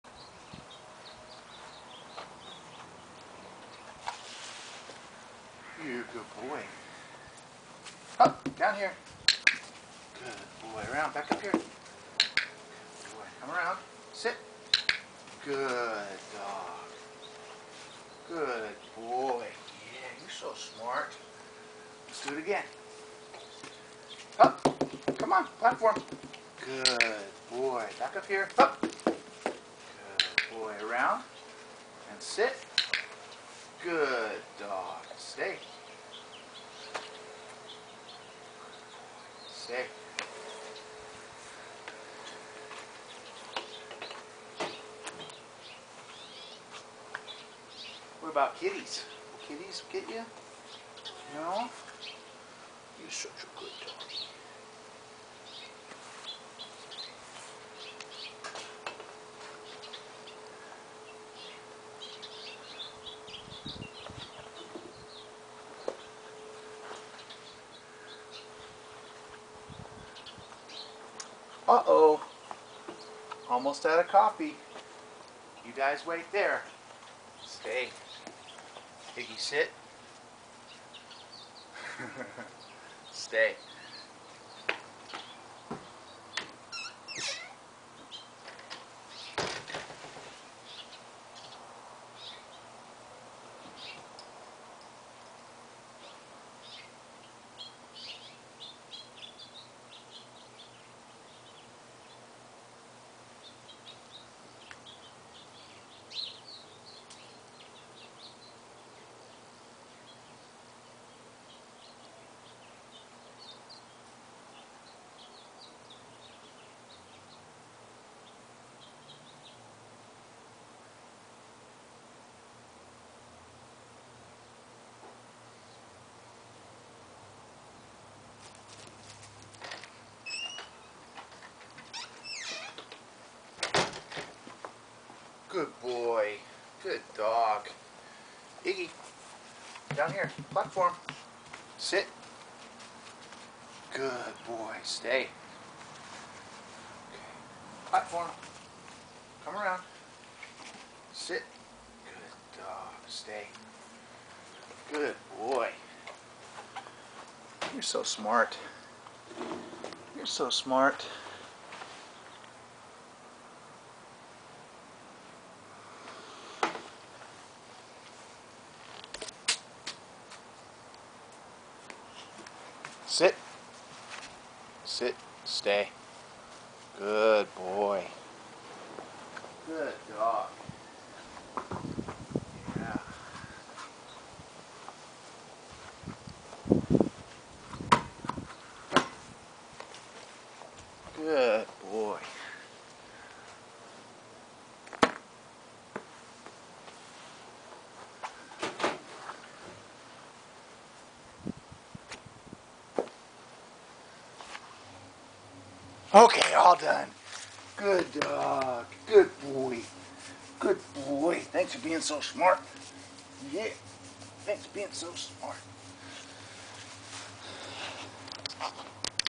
Bullmastiff vs. Cat
Iggy does his hoop and platform routine, with added distraction of flying balls and rowdy cats.